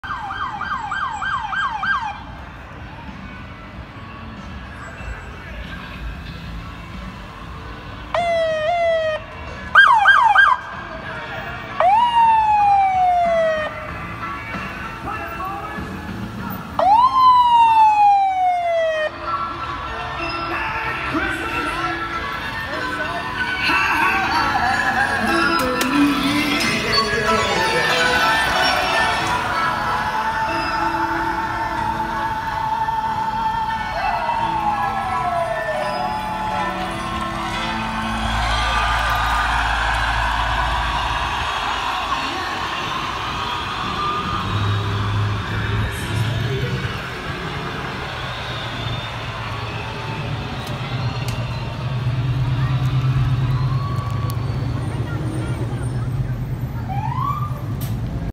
LAPD Christmas Parade 2019 | Gonzaque Village
Los Angeles Police Department 2x 2016 Ford Explorer and a 2002 Chevy Tahoe going around the areas of South Los Angeles tossing sweets to those nearby (as im recording, throws one at me).

Every year, LAPD goes around the city of Los Angeles handing out sugary sweets to children that are in the surrounding areas until Christmas Day.

Watts / Gonzaque Village, Los Angeles - Dec. 4th, 2019